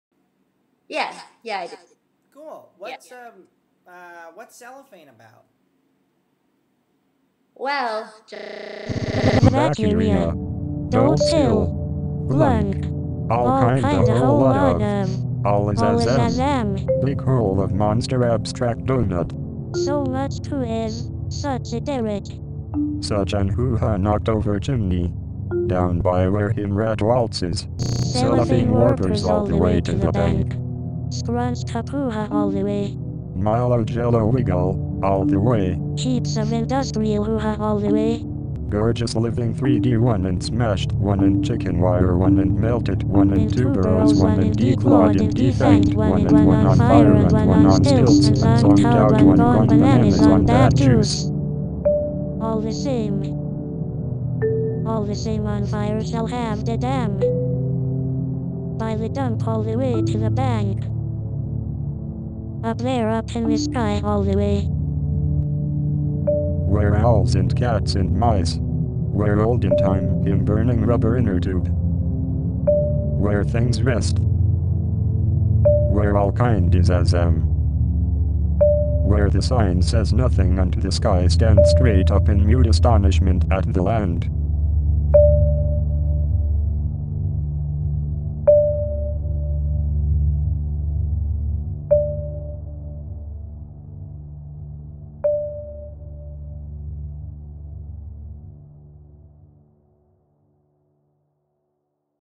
CELLOPHANE TEASER
A revised teaser for CELLOPHANE by Mac Wellman realized by jenny&john